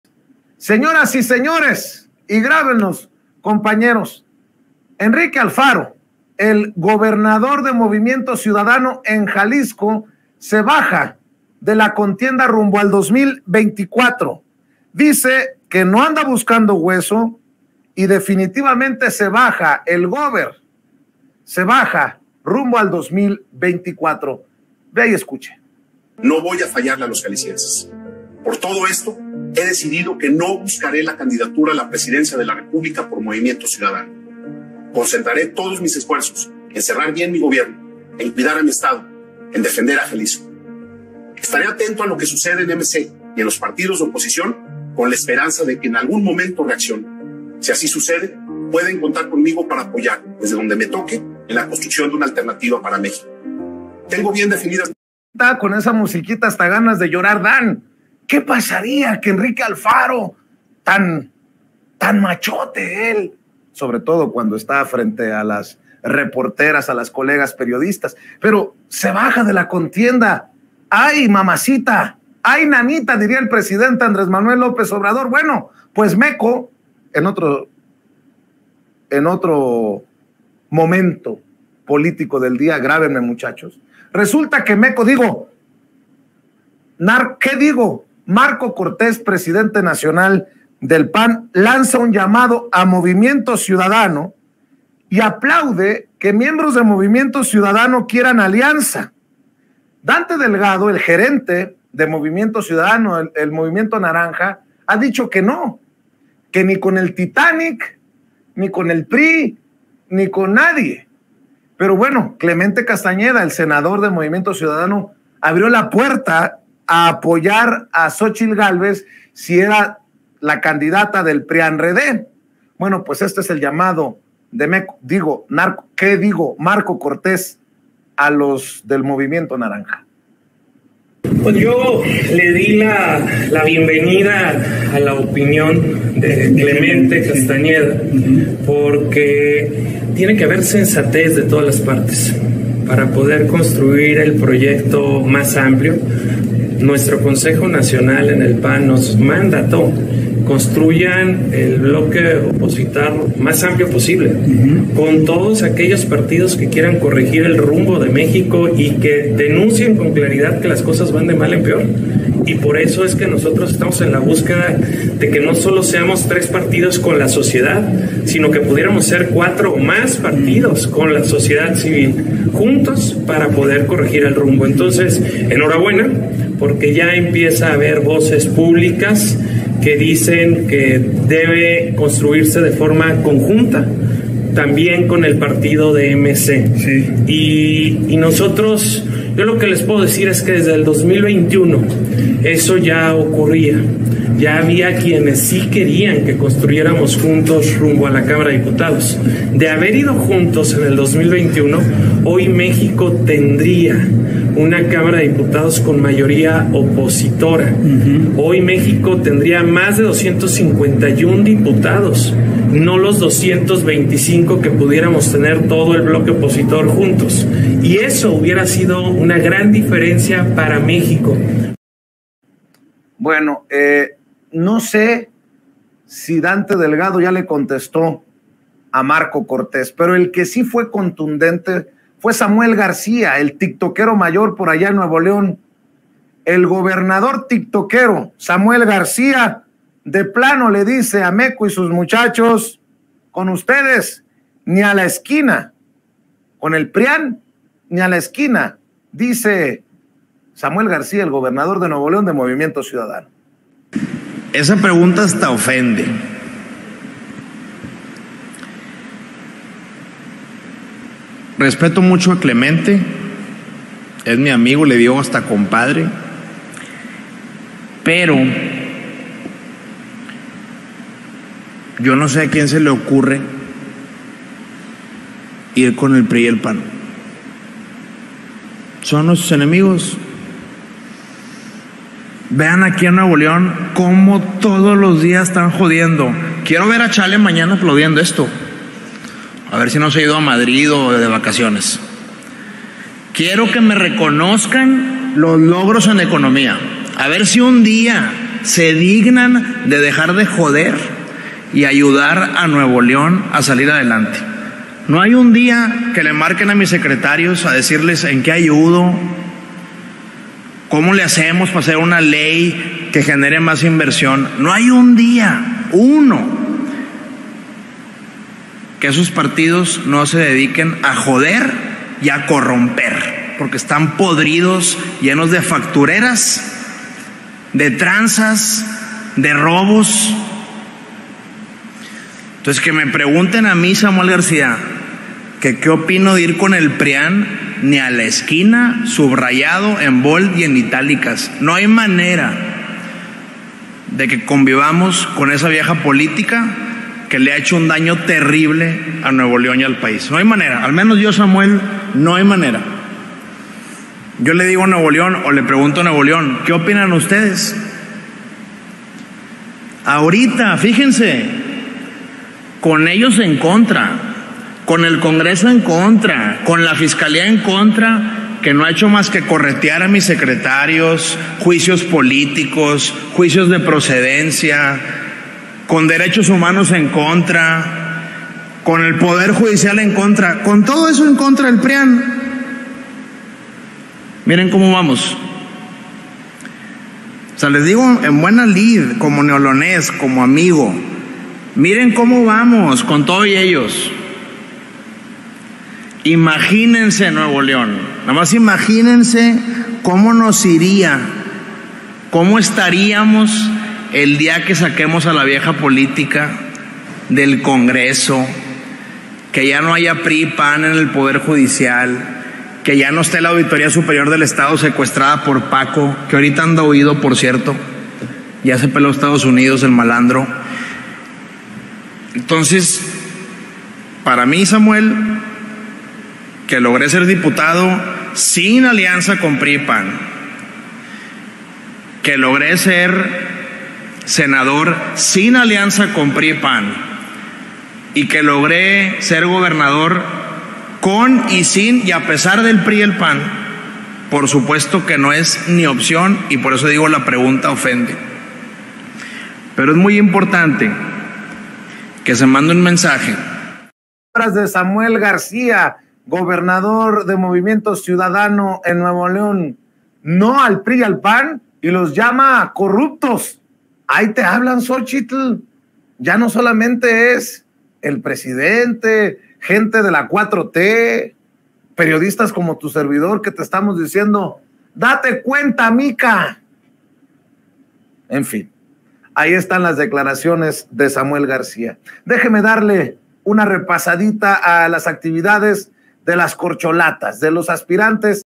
MARKO CORTÉS DICE MOVIMIENTONARANJA APOYARÁ A XÓCHITLGÁLVEZ, SAMUEL GARCÍA LO DESMIENTE
Marko Cortés dice que Movimiento Ciudadano apoyará a Xóchitl Gálvez pero Samuel García lo desmiente